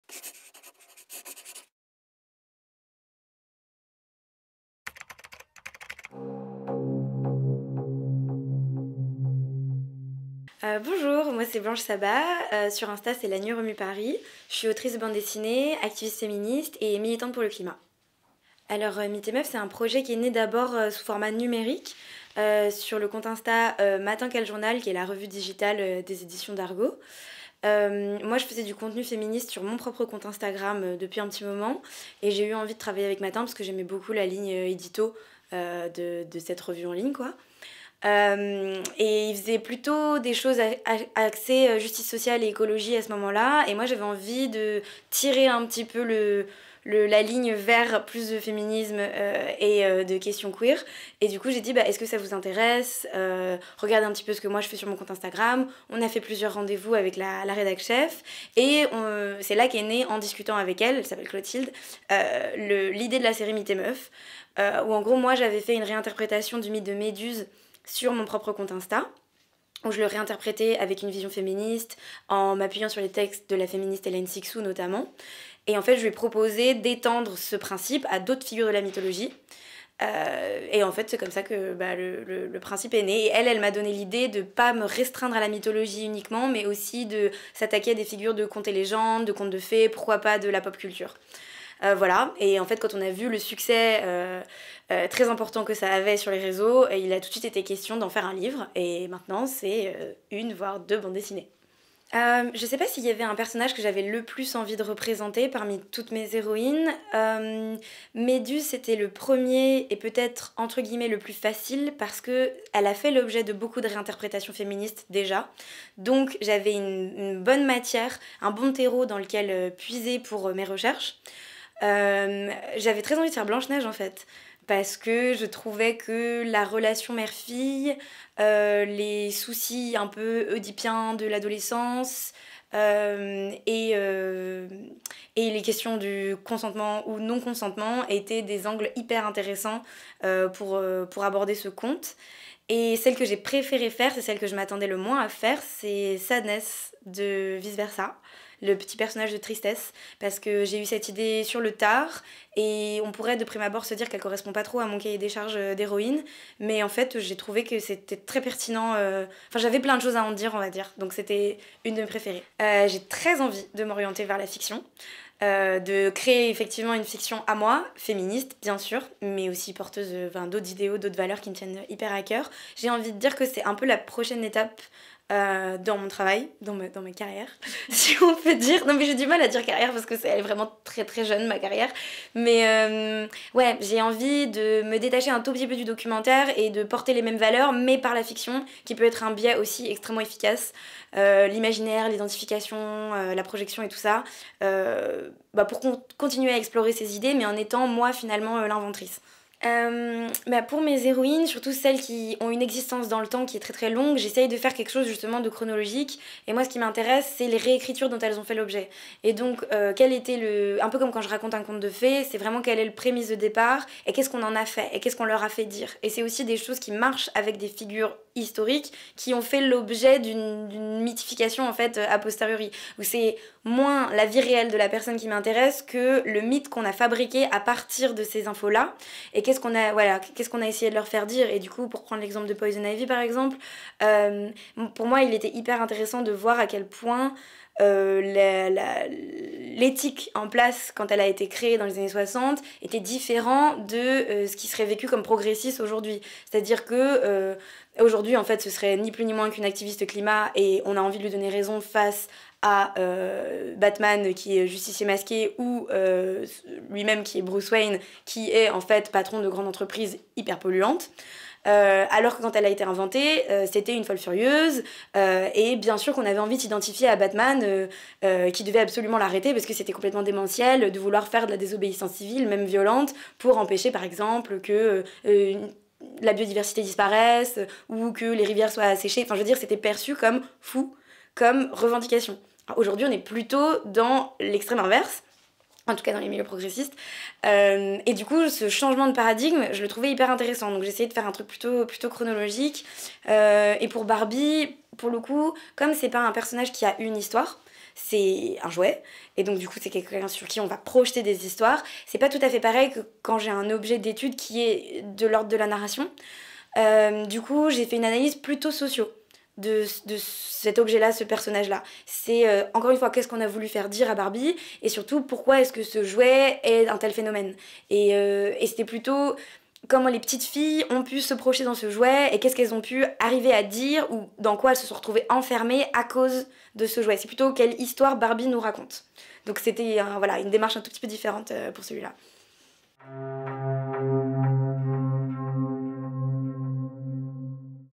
Blanche Sabbah - Mythes & meufs. Vol. 2
Blanche Sabbah  vous présente son ouvrage "Mythes & meufs. Vol. 2" aux éditions Dargaud.